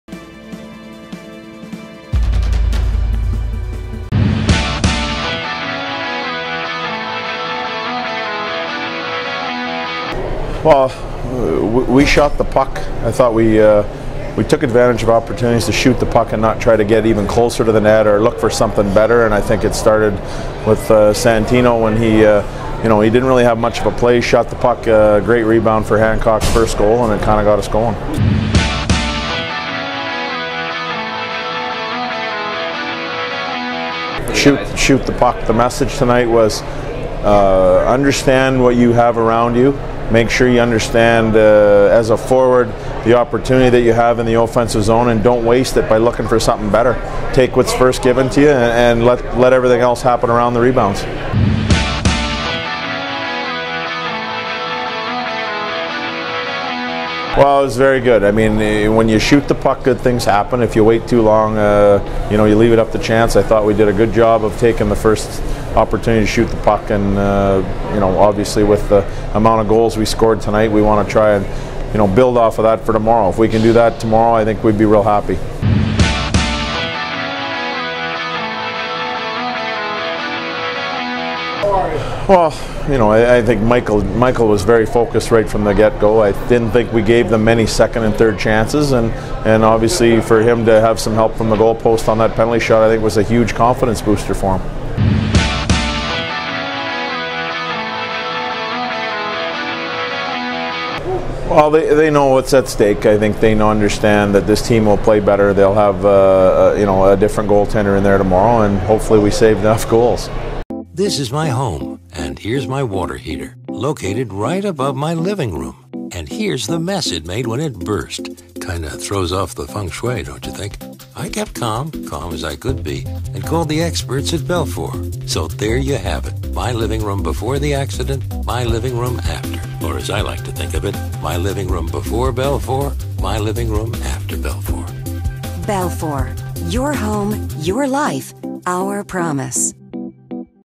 Post Game Comments: Ryan McGill, Jan. 16
Attack head coach Ryan McGill recaps a decisive 8-0 win for his squad over the CHL's top ranked Erie Otters.